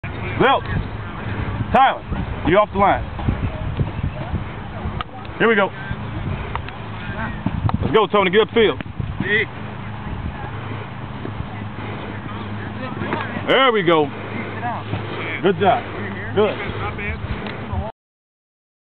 Adam Kobler to Dee Edwards windows 7 on 7 Brentwood Mo
Adam Kobler hits Dee Edwards in the open window in 7 on 7 Brentwood Mo